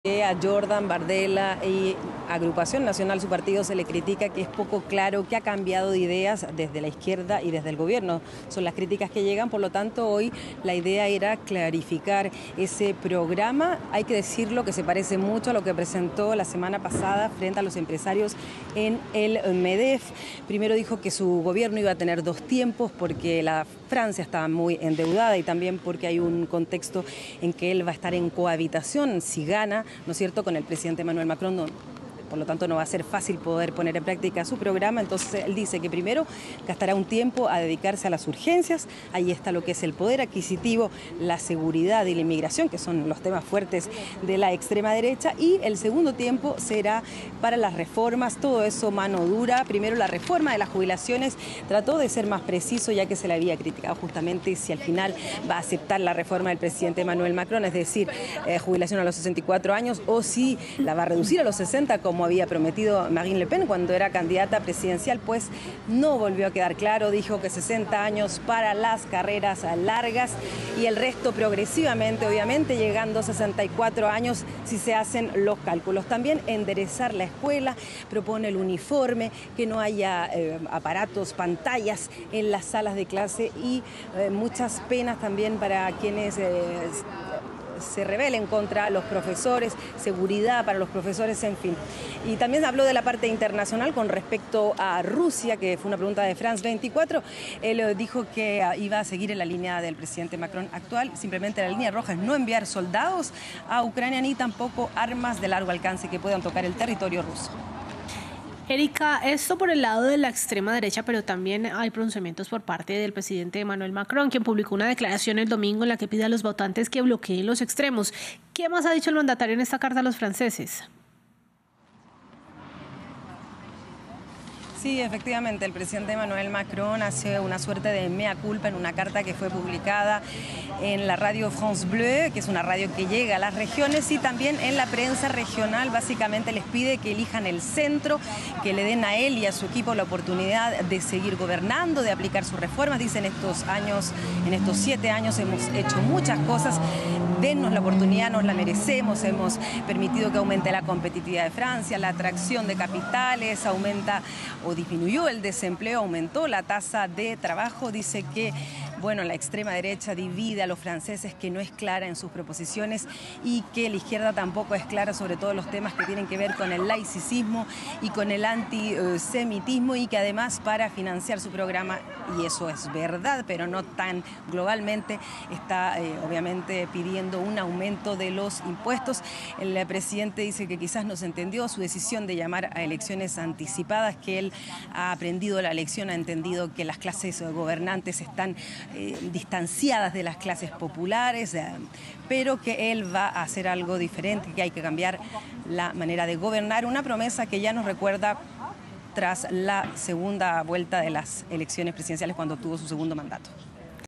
Informe desde París: Bardella presentó plan de gobierno de la extrema derecha • FRANCE 24
La extrema derecha francesa presentó su plan de Gobierno previo a las elecciones del próximo domingo 30 de junio. La izquierda y el Gobierno de Emmanuel Macron han criticado repetidamente a Jordan Bardella y a la extrema derecha por "falta de claridad" en sus ideas. Nuestra periodista Erika Olavarría nos detalló cuáles son los puntos en los que se enfocaría Bardella, mano derecha de Marine Le Pen y aspirante al cargo de primer ministro.